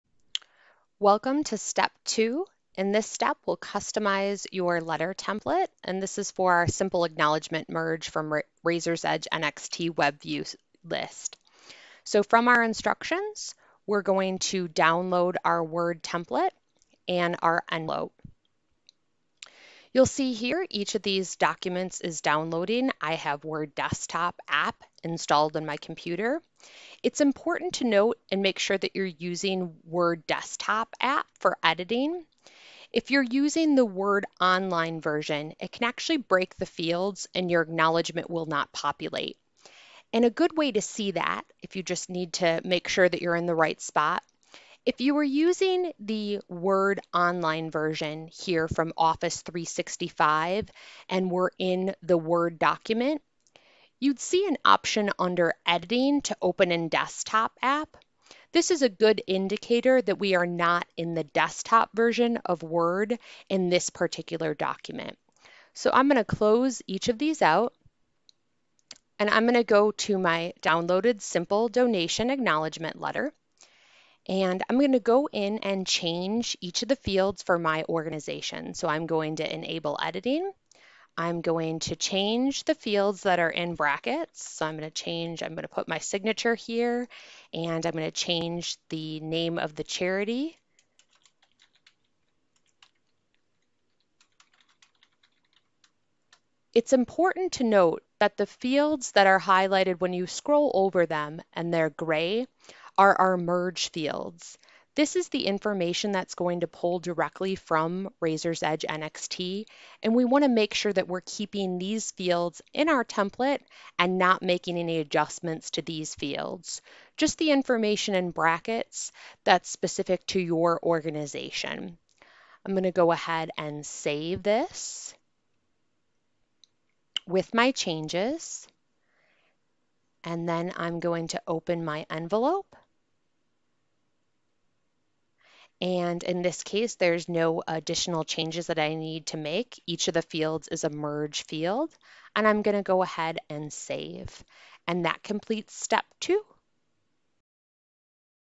Step 2 Customize your letter template
Create an acknowledgement letter using the webview list in Blackbaud Raiser's Edge NXT and a simple merge in Microsoft Power Automate